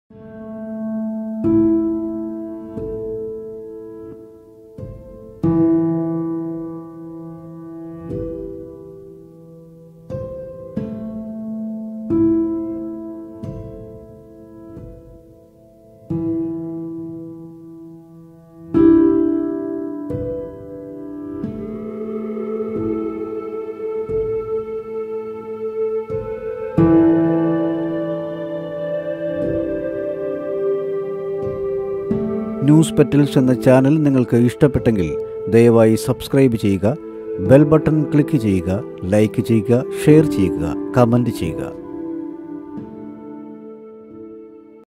Malayalam Actress Monisha Last Journey Photo Album | News Petals | Pathrathaalukaliloode
Actress Monisha acted in so many Malayalam movies like Cheppadividya, kamaladalam, adhipan, nakhakshathangal, perumthachan, etc